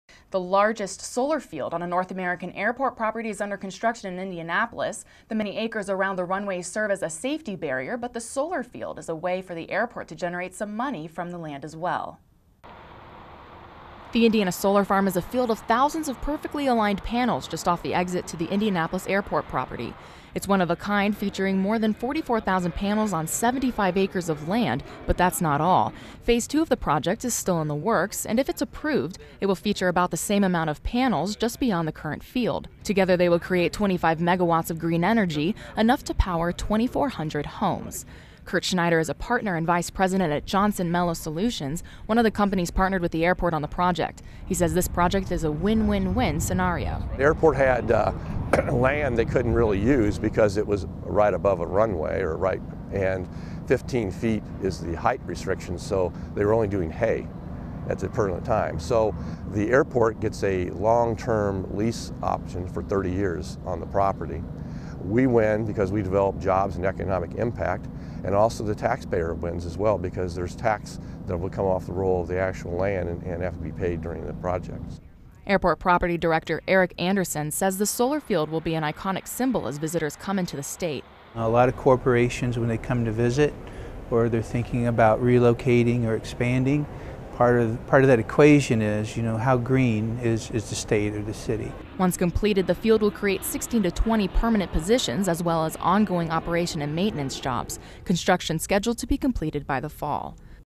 Indianapolis Airport Solar Farm To Be Completed This Fall
Full Story WTIU's Alex Dierckman reports on the Indianapolis Airport's new Solar Farm and how it will impact the community.